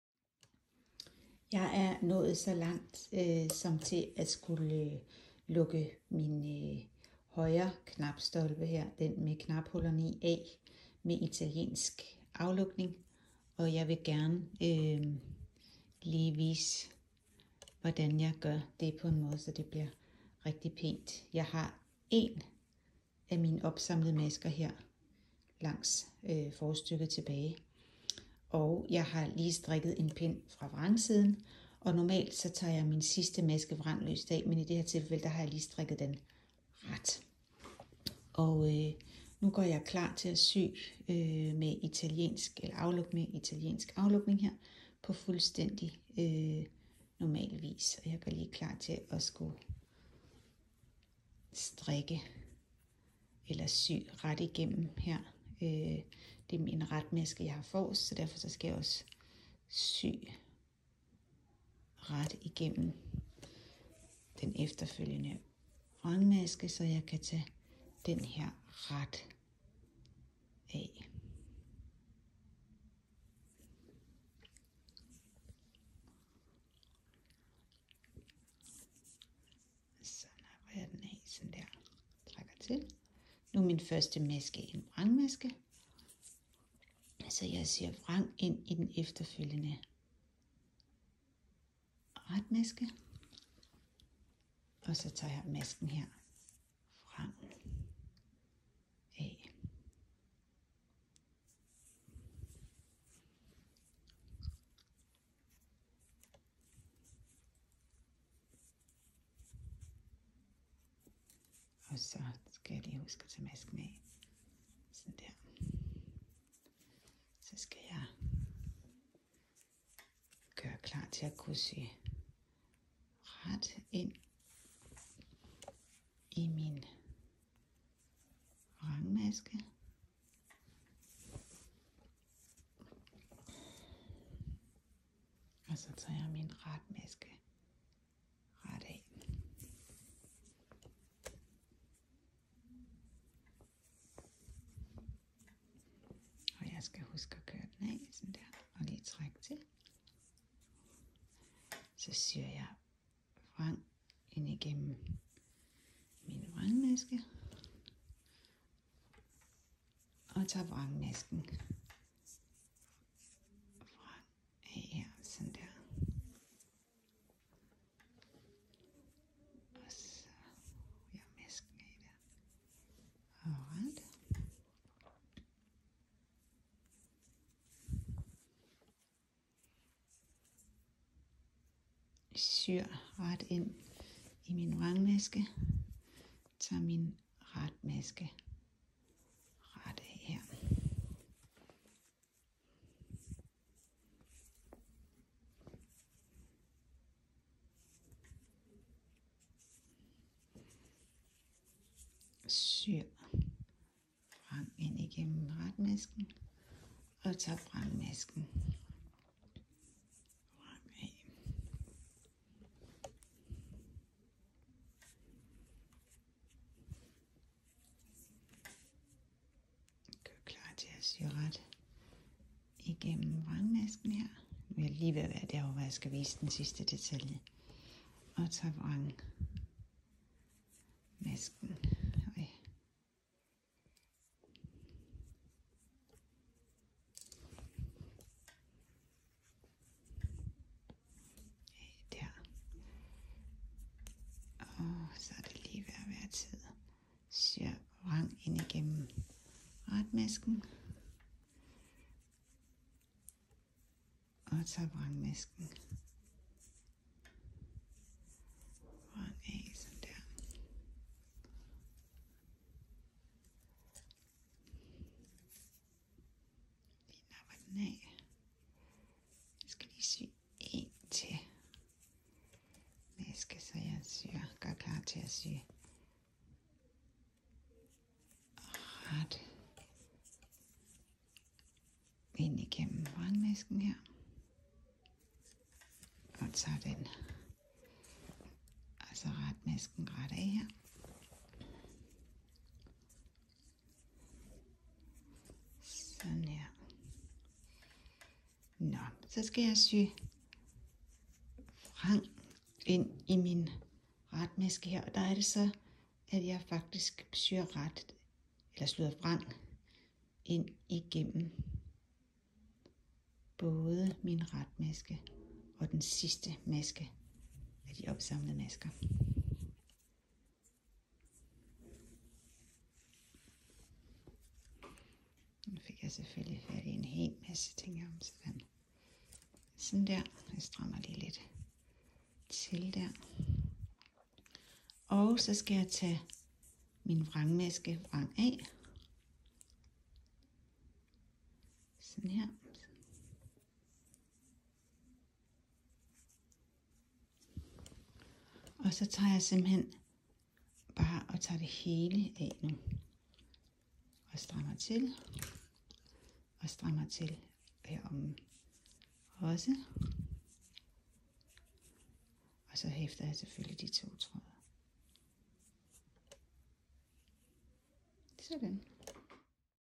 Linea Top Placket: Italiensk aflukning af knapstolpe/ Binding of the placket - Italian bind off
I videoen viser jeg, hvordan jeg aflukker knapstolpens masker med italiensk aflukning.

Obs! I videoen siger jeg, at jeg strikker den sidste maske på den sidste pind før aflukning ret. Det er en fejl, den skal strikkes vrang.

The video shows how to bind off the placket with the Italian bind-off method.